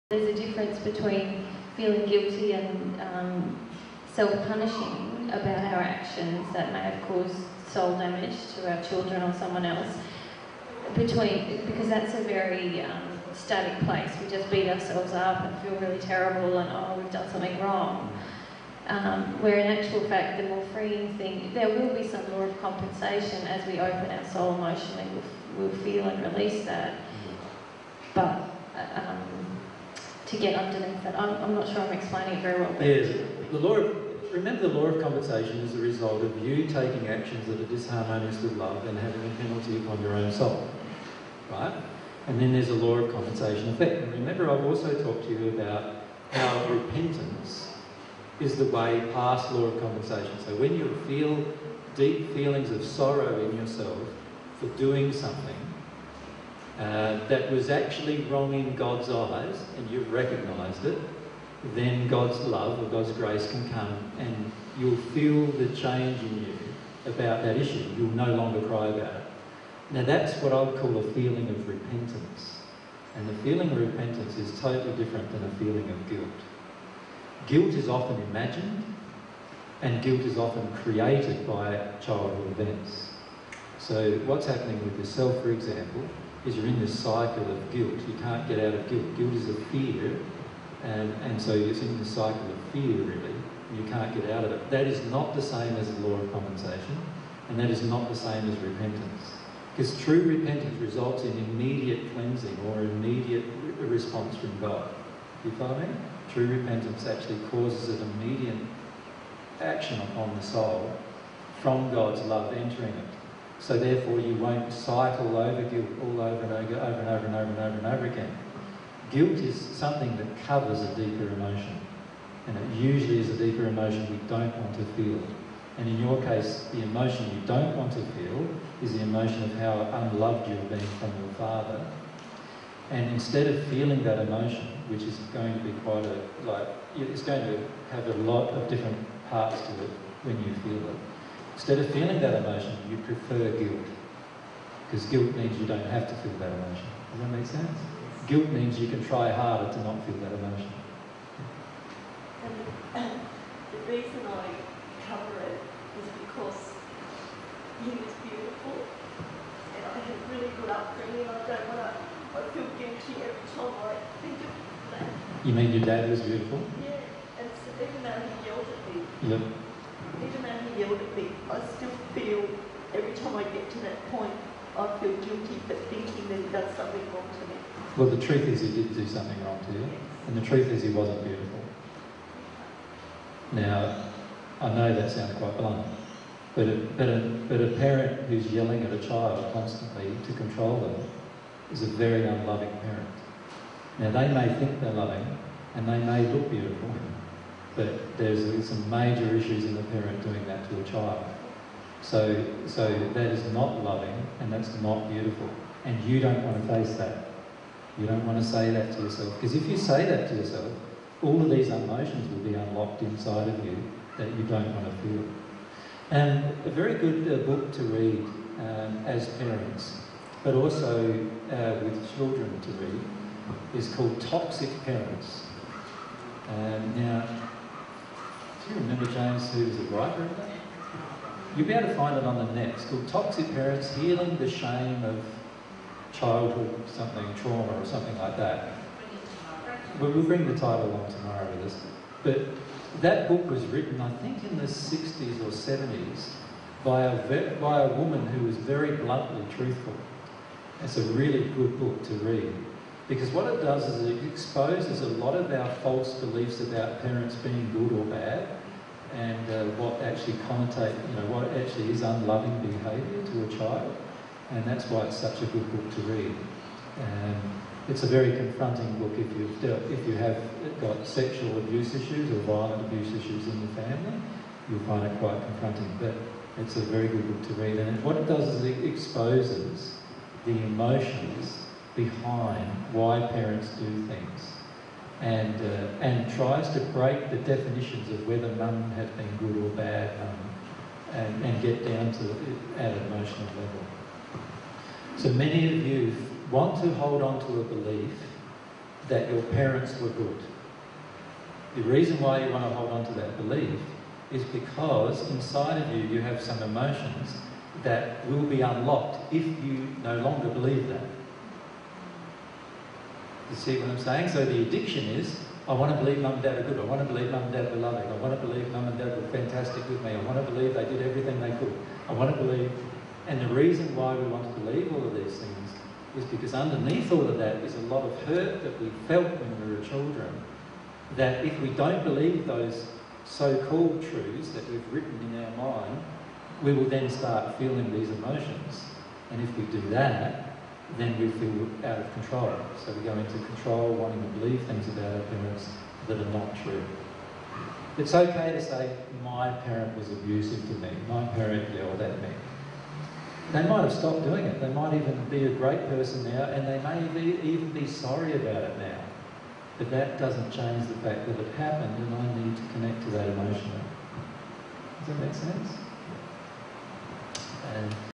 Jesus & Mary on Toxic Parents and Childhood Guilt
Jesus and Mary discuss toxic parents and the effects they have on their children.

This is a section of the presentation by Jesus entitled "20090307 Human Relationships - Parenting Children S1P2".

dtstart:014638:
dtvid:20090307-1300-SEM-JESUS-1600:
dtmd5:3d2265b08582622ffd7bf6691b8963da: